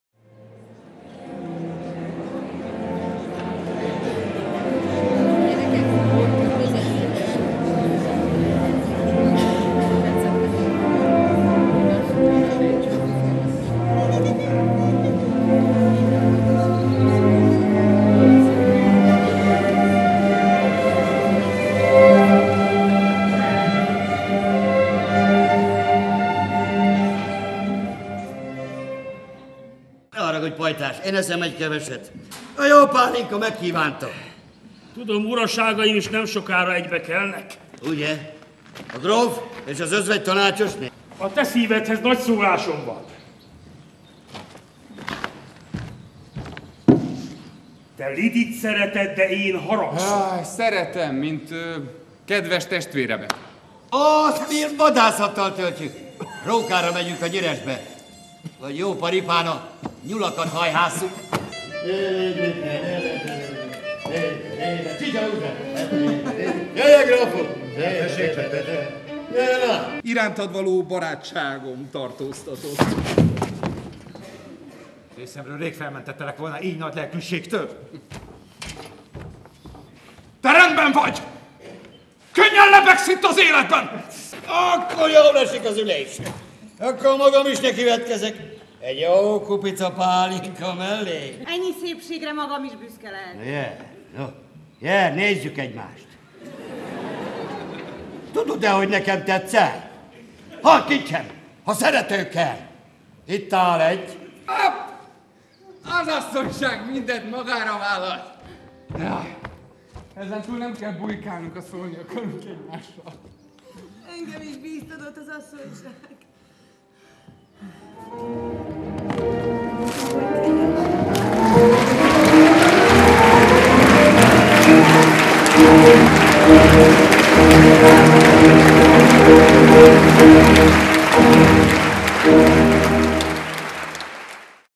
Kisfaludy Károly: Csalódások (vígjáték) - 2013. július 2.
Városmajori Szabadtéri Színpad
A Városmajori Színházi Szemle programjában.
A Veszprémi Petőfi Színház előadása.